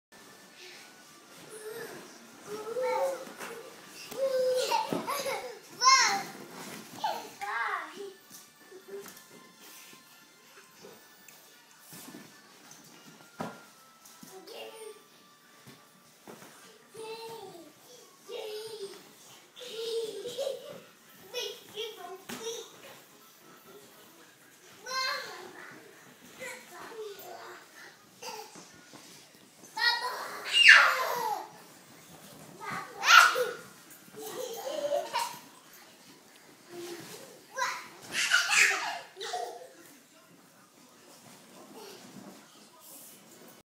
Ace and Reilly playing with giant soccer and beach ball
Running around and playing